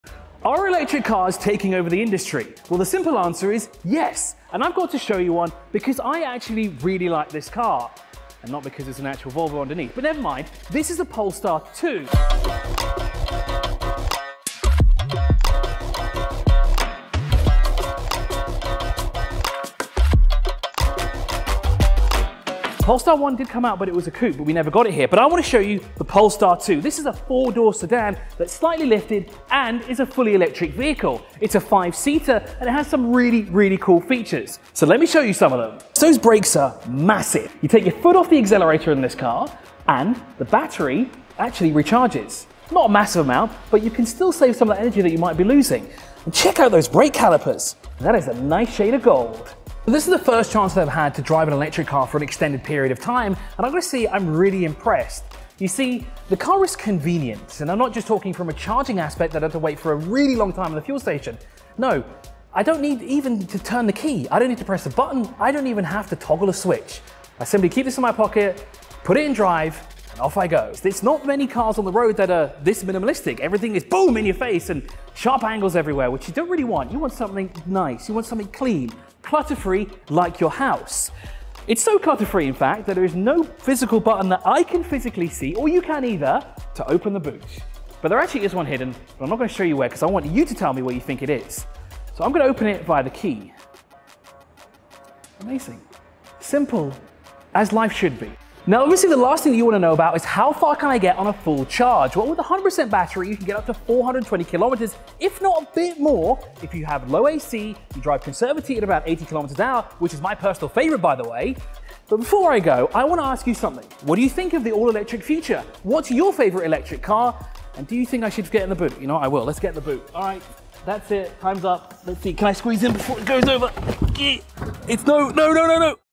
Step into the Future of Cars with Polestar 2
⚡🌟 Step into the Future of Cars! 🚗 @polestarcarsuae

The electric car revolution is in full swing, and the market is buzzing with innovation. But how does a standout star emerge from the electric galaxy?

Polestar, with its daring approach, has found the answer in minimalism – a design so sleek, it's impossible to ignore. But make no mistake, there's nothing basic about it.

The Polestar 2 has officially arrived in the region and with a name that's as easy to remember as it is unforgettable, this car is your ticket to an electrifying adventure. Buckle up, hit the accelerator, and feel the future as grip the steering wheel.

🚗!انطلق نحو مستقبل السيارات ⚡🌟

ثورة السيارات الكهربائية في تطور مستمر، والسوق يشهد ازدهاراً كبيراً، لكن كيف يمكن أن تتميز السيارات الكهربائية في ظل هذا التنافس الكبير؟

سيارة بولستار الجديدة أجابت عن التساؤل بالتصميم البسيط والمميز الذي لا يمكن تجاهله

رسمياً وصلت سيارة بولستار الكهربائية الجديدة إلى المنطقة مع اسم سهل ومميز لا يمكن نسيانه، اربط حزام الأمان وانطلق إلى المستقبل مع بولستار الجديدة.

شاركنا رأيك في التعليقات، هل أعجبتك السيارة؟